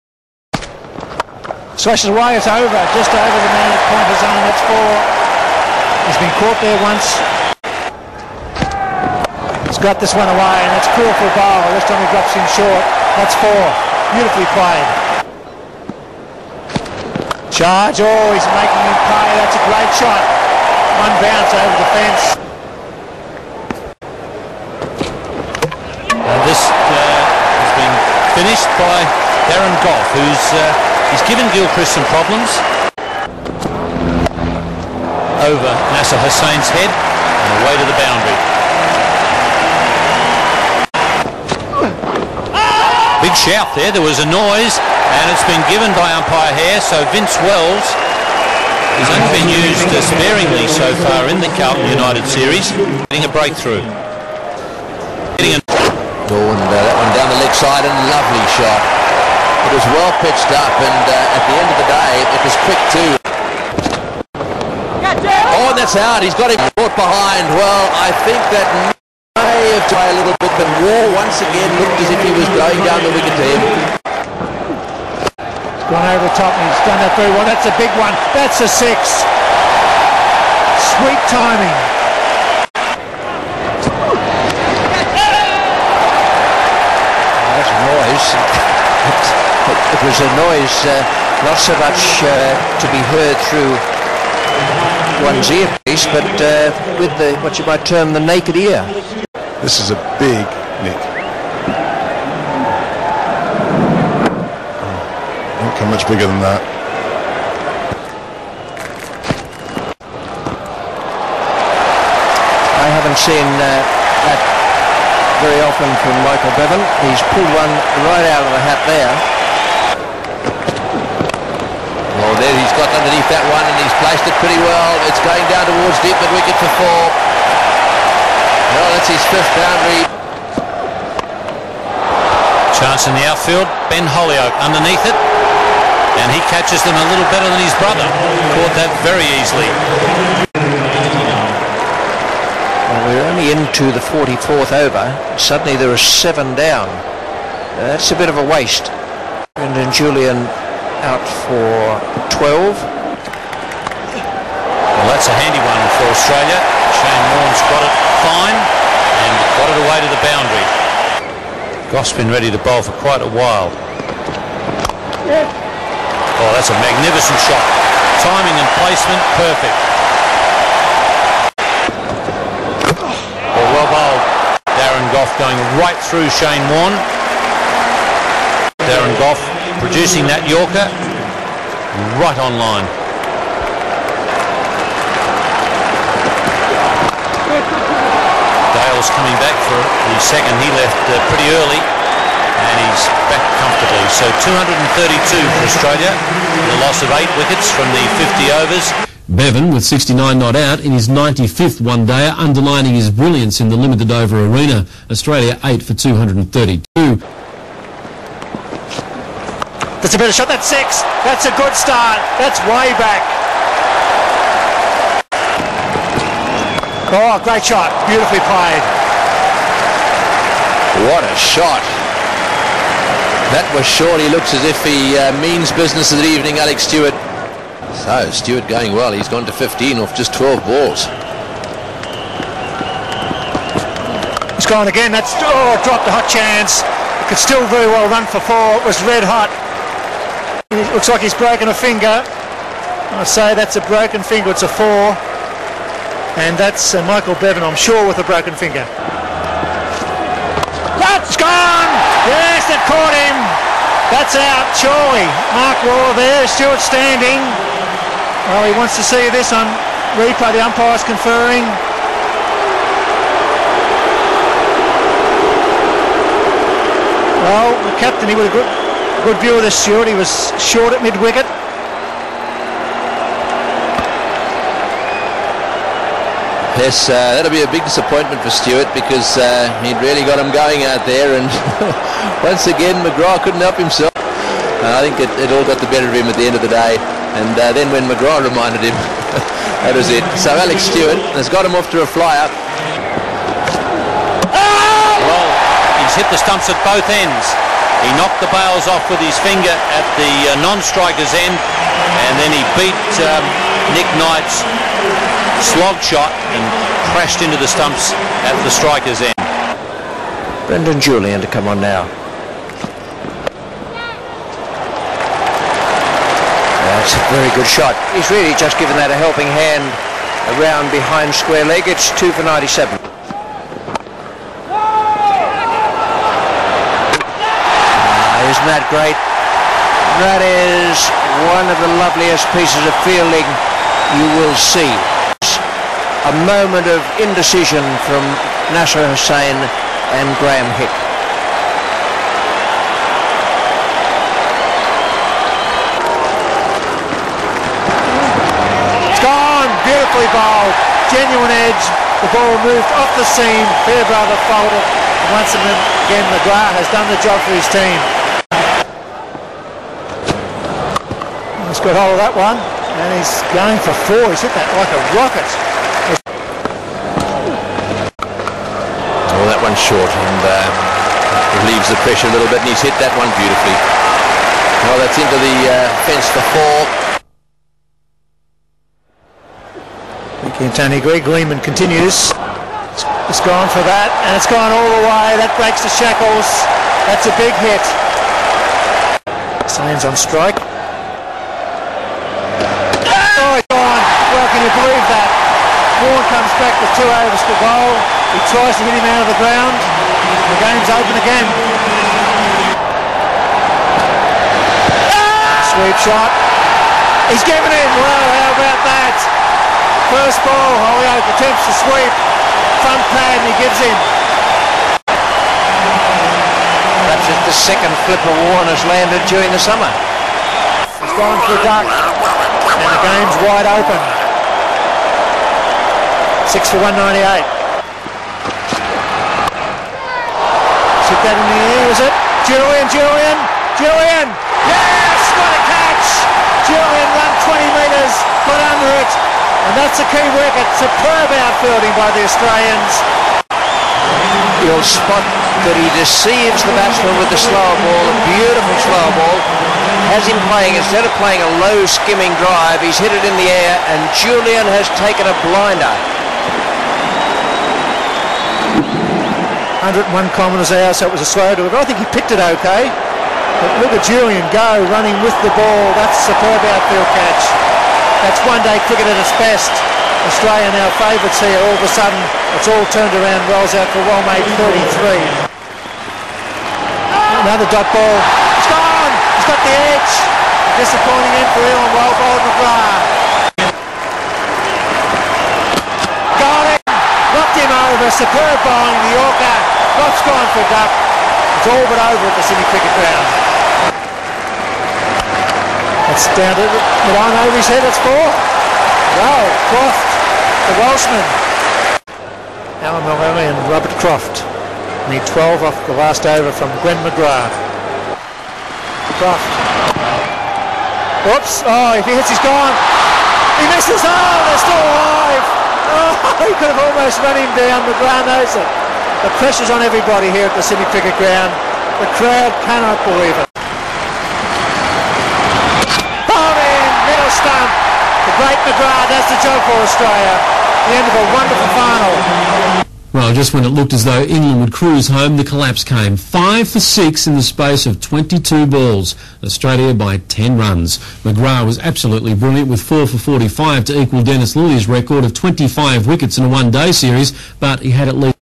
AUSTRALIA vs ENGLAND, 1998/1999 WSC 1st FINAL, SCG
England secured the right to meet the Australians in the first final after a long series in 1998/1999. The first final was played at the Sydney Cricket Ground.

TEAMS

AUSTRALIA: (6) Mark WAUGH, (18) Adam GILCHRIST, (14) Ricky PONTING, (25) Darren LEHMANN, (30) Damien MARTYN, (12) Michael BEVAN, (20) Shane LEE, (8) Brendon JULIAN, (23) Shane WARNE (C), (24) Adam DALE, (11) Glenn McGRATH

ENGLAND: (15) Nick KNIGHT, (1) Alec STEWART (C), (10) Graeme HICK, (14) Nasser HUSSAIN, (6) Neil FAIRBROTHER, (17) Vince WELLS, (11) Adam HOLLIOAKE, (5) Mark EALHAM, (4) Robert CROFT, (8) Darren GOUGH, (16) Allan MULLALLY